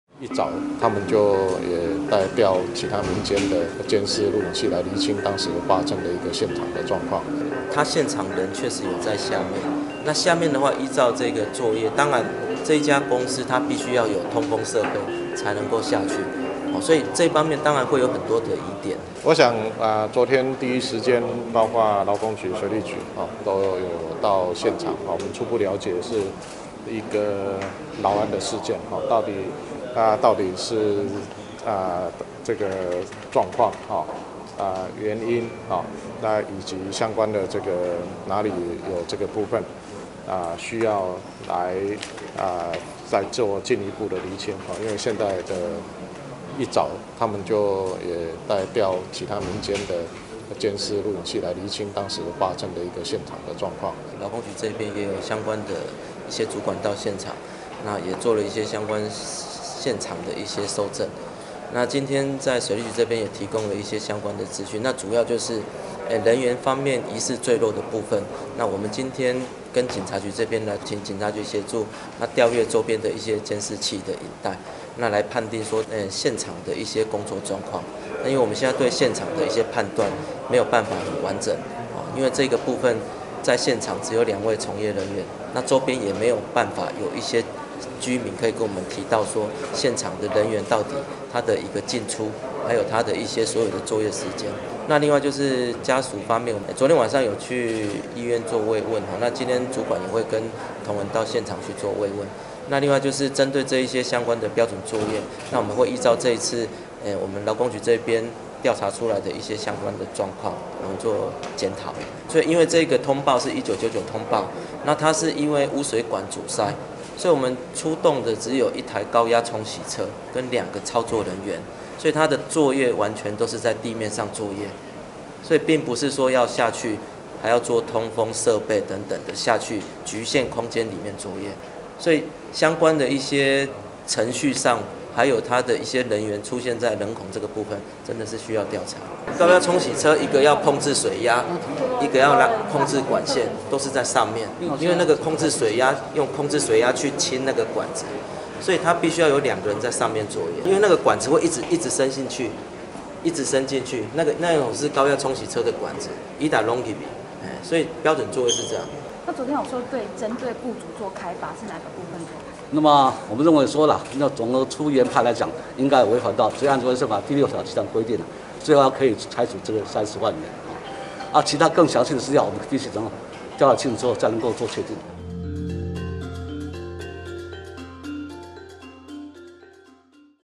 高雄下水道工安事故2死 陳其邁︰調監視器釐清案情
高雄市鹽埕區昨晚發生工安意外，2工人不明原因在下水道受困、救出送醫不治；高雄市長陳其邁今指出，第一時間勞工、水利等單位均到場了解，目前正調閱民間監視器釐清過程。